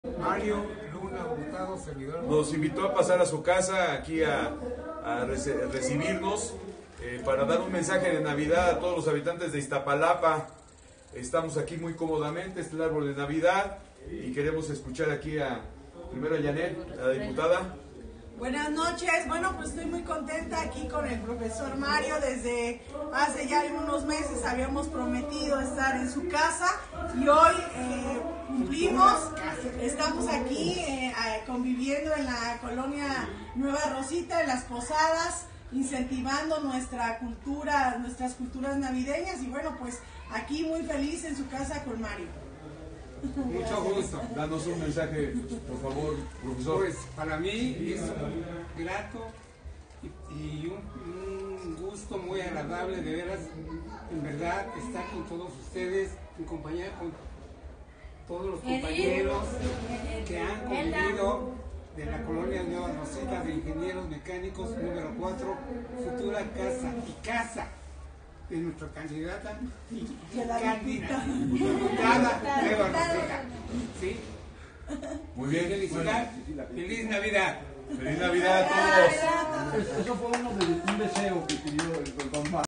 DiputadaEnTuCasa, col. Nueva Rosita. Posada Tradicional (Diciembre 22, 2016)
Cerramos el año con PosadasTradicionales por toda la delegación Iztapalapa, celebrando con nuestra gente.
En todas conjuntamos los elementos más tradicionales para fomentar en nuestras niñas y niños la preservación de nuestras tradiciones.